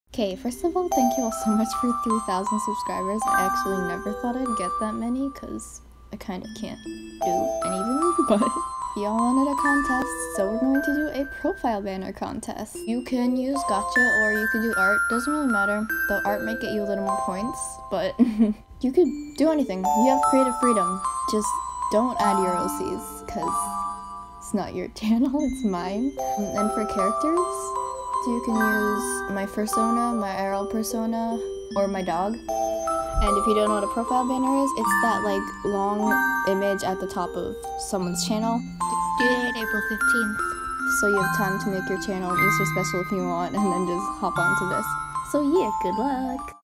~ Profile Banner Contest ~ 3k Contest ~ !!!!READ DESC!!! ~
AAAAA TYSM FOR 3K OML I NEVER THOUGHT I’D EVER GET THIS MANY SUBS LIKE EVER SO TY ALL SMMMM AAAAA ILY ALLLLLLLLL 💛💚💛💚💛💚💛💚💛💚💛💚💛💚💛💚💛💚💛💚💛💚💛💚💛💚💛💚💛💚

Now If You’re Interested In The Contest Please Continue Reading Cause I Changed Some Stuff Lmao~

Rules:

~Get It Done Before The Due Date
~I Think April 15th Is Too Soon And Would Like To Give More Time So April 18th Is The New Deadline :D
~Use One (Or All) Of The Characters Listed In The Video (Either My Fursona, Irl Persona, Or My Doggo)
~DO NOT INCLUDE YOUR OC (It’s Not Your Channel Lmao)
~A YouTube’s Profile Banner Must Be 2560 x 1440 So Please Make Sure Your Image Is The Correct Size TwT
~Use The Hashtag OmnisNew3kBanner When Uploading Your Video/Image
~Have Fun Rwawr

Cyaaaaa